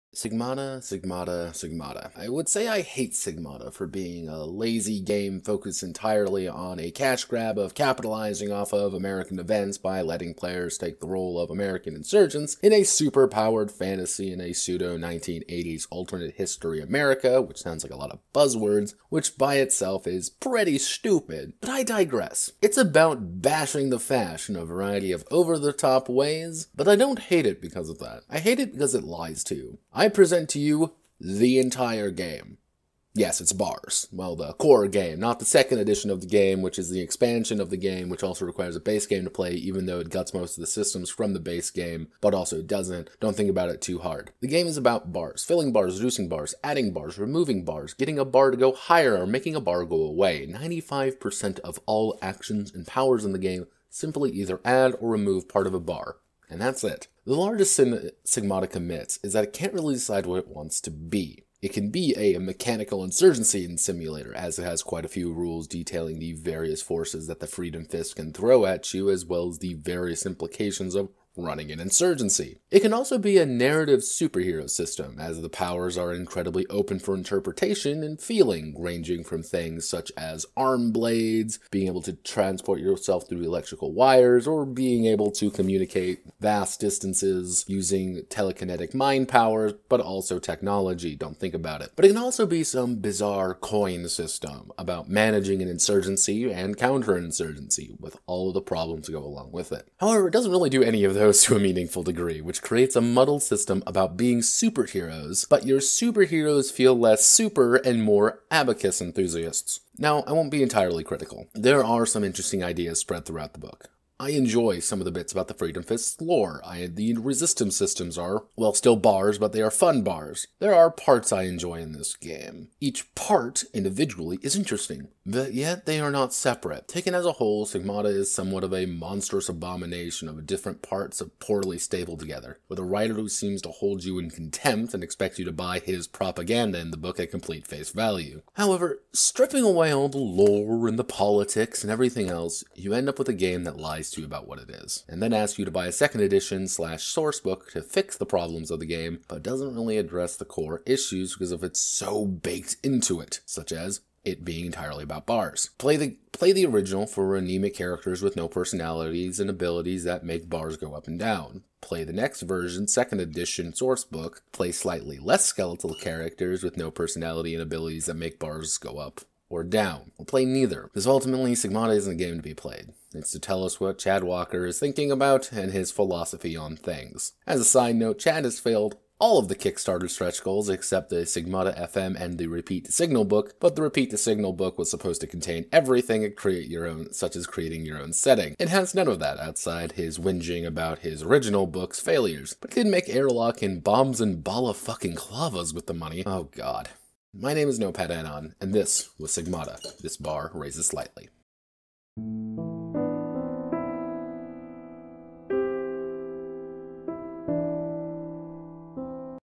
Notepad's Little Opinion on Sigmata: TSKF/RTS is about 3 Minutes
My Opinion on Sigmata: This Signal Kills Fascists & Repeat the Signal, a condensed version of the livestream.
As a side note, I have a small personal history with Chad Walker, the writer. I also think he's a charlatan who huffs his own excrement for his personal enjoyment. I am biased, but I'm not wrong about any part of it.

Ending Music from: Ben Sounds

This is entirely my own opinion and nothing else, the stream can be found in the Curious Case Playlist.